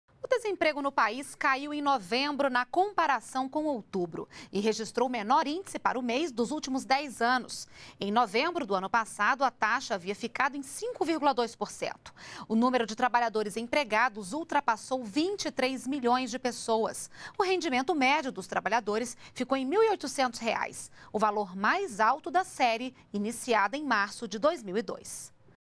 Número de pessoas desempregadas diminui no mês de novembro
NBR NOTÍCIAS - 21.12.12: O desemprego no Brasil caiu em novembro na comparação com
outubro e registrou o menor índice dos últimos 10 anos. Em novembro do ano passado, a taxa havia ficado em 5,2%. O número de trabalhadores empregados ultrapassou 23 milhões de pessoas./O rendimento médio dos trabalhadores ficou em R$ 1.800, o valor mais alto da série iniciada em março de 2002